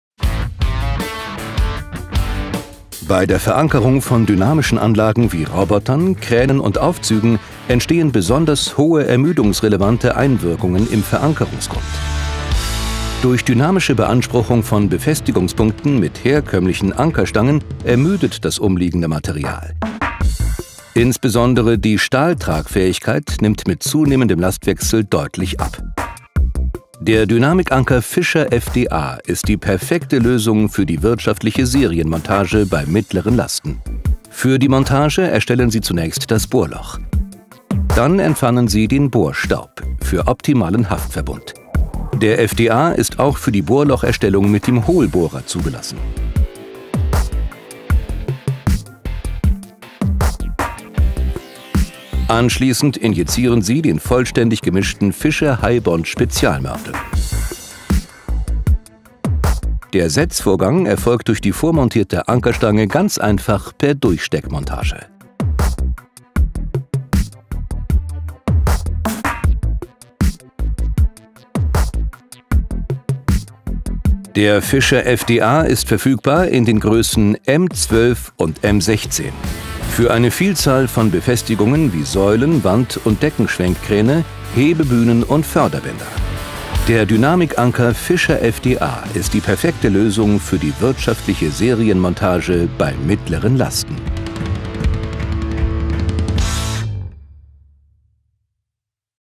Dynamikanker FDA
Der fischer Dynamic-Anker FDA wird mit dem Highbond-Spezialmörtel FIS HB verarbeitet. Der Anker verfügt über eine allgemeine bauaufsichtliche Zulassung für Dynamikanwendungen in gerissenem Beton. Durch seine Konengeometrie eignet er sich, um dynamische Lasten mittleren Lastniveaus im Innenbereich zu befestigen. Die vormontierte Ankerstange und die Durchsteckmontage ermöglichen einen zügigen Arbeitsfortschritt und prädestinieren ihn für eine wirtschaftliche Serienmontage. Mit dem Dynamic-Anker FDA werden z.B. Hebebühnen und Fördersysteme befestigt.

Anwendungen:

- Pkw-Hebebühnen
- Förderbänder
- Industrieroboter
- Kooperations-Roboter und Anlagen
- Führungsschienen von Aufzügen

Ihre Vorteile im Überblick:

- Mittleres Lastniveau für eine Vielzahl von Anwendungen.
- Vormontierte Ankerstange für eine schnelle Montage.
- Geprüfte Sicherheit durch allgemeine bauaufsichtliche Zulassung für unendlich viele Lastwechsel.
- Einfache Durchsteckmontage sorgt für hohe Wirtschaftlichkeit, vor allem bei Serienmontage.
- Straffes Sortiment in den Größen M12 und M16 in galvanisch verzinktem Stahl.
- Kleine Randabstände erlauben Befestigungen am Plattenrand.

Sind wir schon auf Twitter vernetzt? /fischer_global